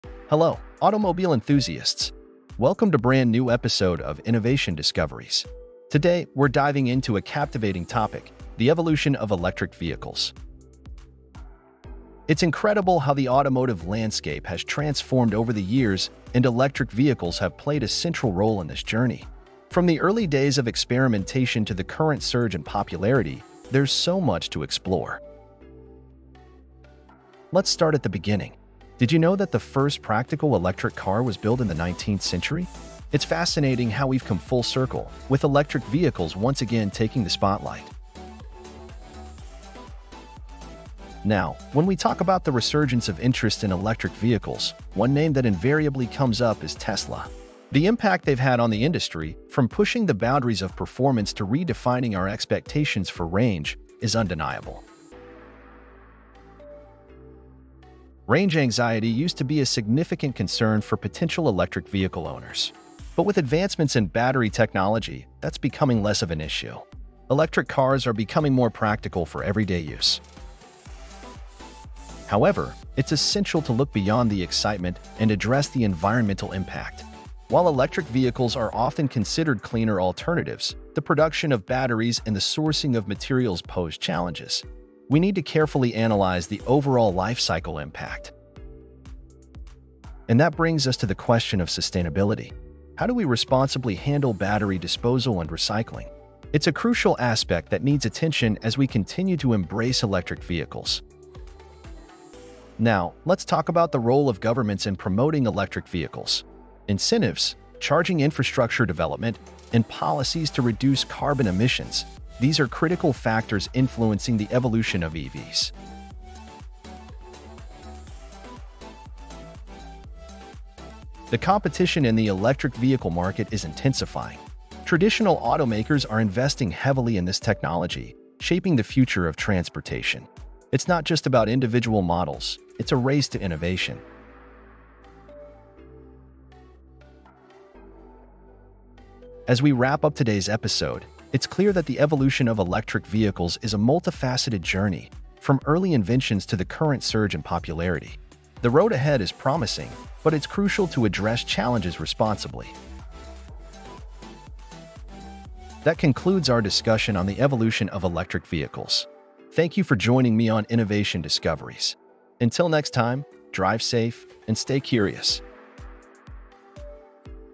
Evolution of Electric Vehicles
Welcome to Episode 01 of "Innovation Discoveries" - The Evolution of Electric Vehicles! 🌐

🔍 **Episode Highlights:**
   📅 From 19th-Century Origins to the Present Day
   🚀 Tesla's Impact on the EV Revolution
   🔋 Addressing Range Anxiety & Battery Technology
   🌱 Sustainability Challenges & Solutions
   🌍 Government's Role in Shaping the EV Landscape
   🏁 The Intensifying Competition - Race to Innovation

🎙️ **Expert Insights:**
   Hear from industry experts as we discuss the transformative impact of electric vehicles on our daily lives, the environment, and the future of transportation.

Don't miss out on future episodes! Let's drive towards a sustainable and electrifying future together!

   Engage with us in the comments below - share your thoughts on the evolution of electric vehicles and what you'd like to discover in future episodes!